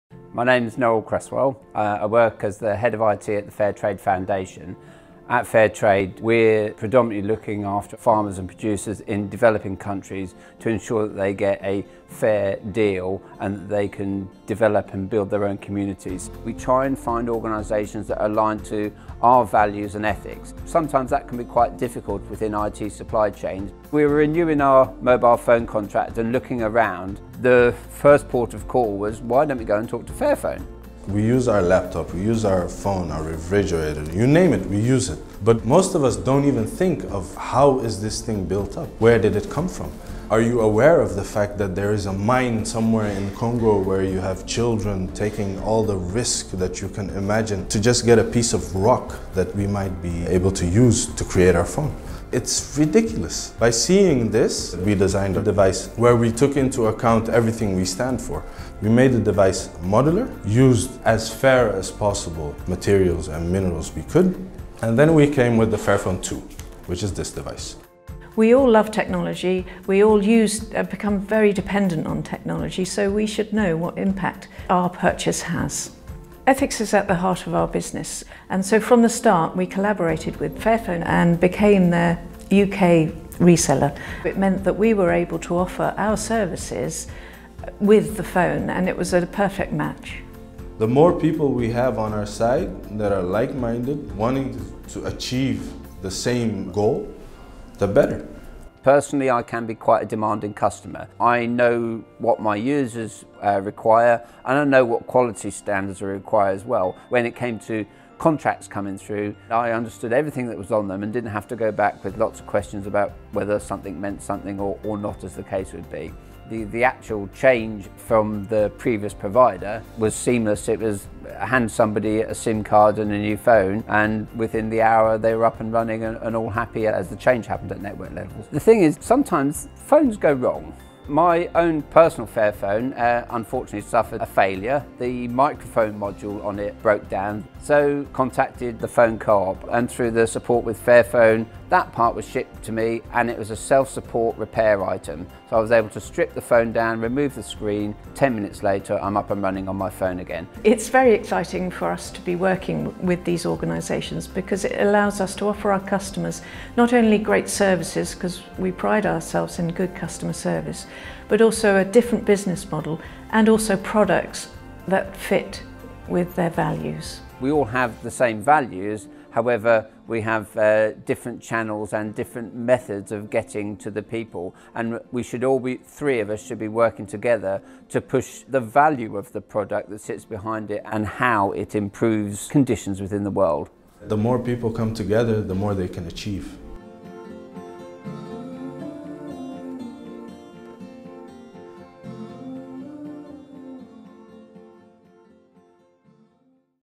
Together we make a difference
It is very exciting for The Phone Co-op to work with organisations like Fairtrade and Fairphone. Together we make a difference, together we can build a better world.